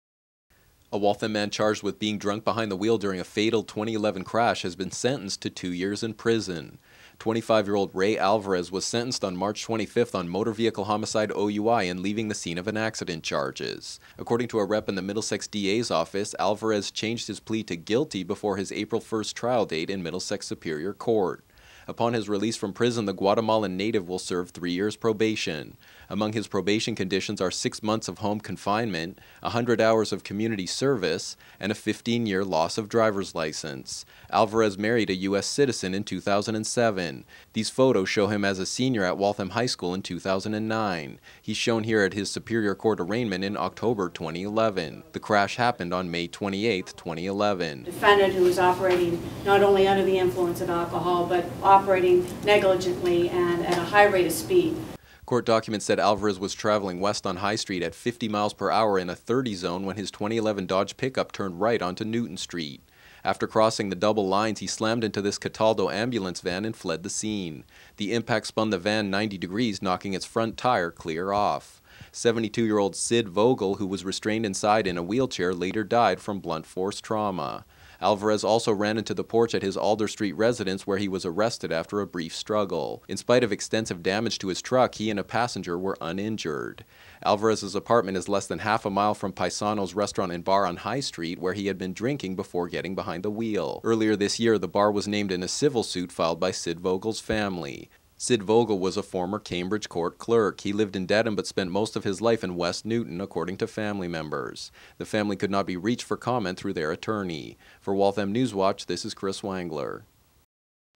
Waltham Drunk Driver Sentenced to 2 Years
Waltham Newswatch, April 4, 2013
After changing his plea, Ray Alvarez, 25, was sentenced on March 25 in connection with the May 2011 crash that killed 72-year-old Irving "Sid" Vogel. He will serve two years on the motor vehicle homicide / OUI charge and two years concurrently on a charge of leaving the scene of a collision causing personal injury and death. A third charge earned him three years' probation following his release.